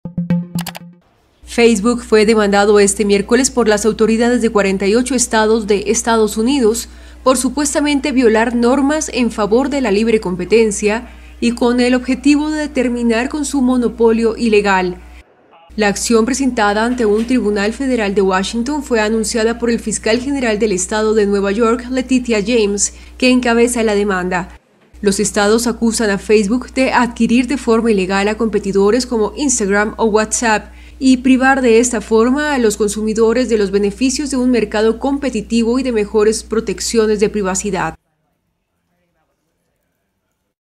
Autoridades de EUA demandan a Facebook por violar las normas antimonopolio
Las autoridades de 48 estados de EE.UU. demandaron este miércoles a Facebook por supuestamente violar normas en favor de la libre competencia y con el objetivo de terminar con su "monopolio ilegal".

¡Visítenos!